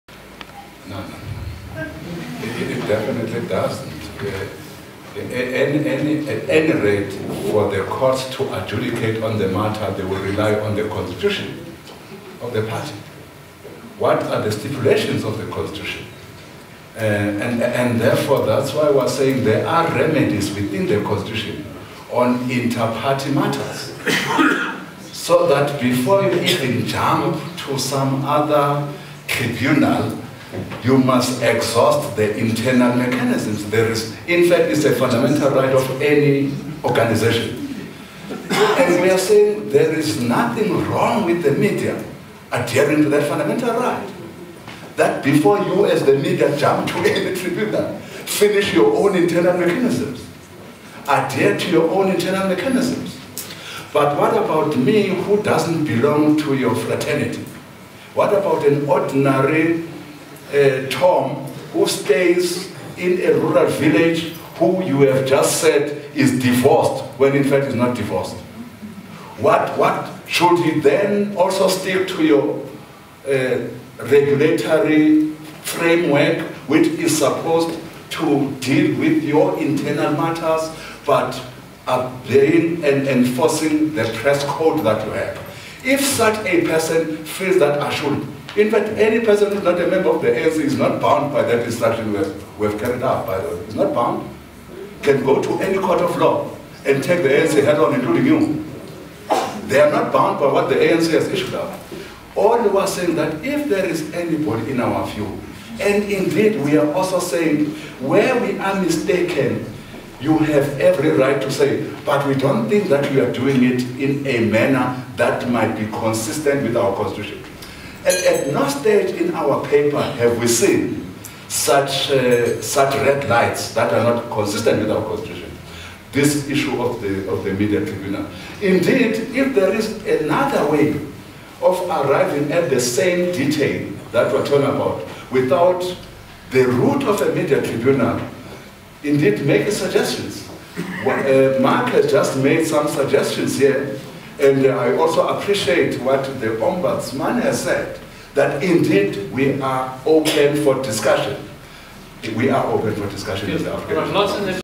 Jackson Mthembu at the M&G Critical Thinking Forum on media freedom
ANC spokesman Jackson Mthembu, responding to a challenge from the floor on the ANC policy that none of its members may turn to the courts, yet wants an "independent" tribunal for the media.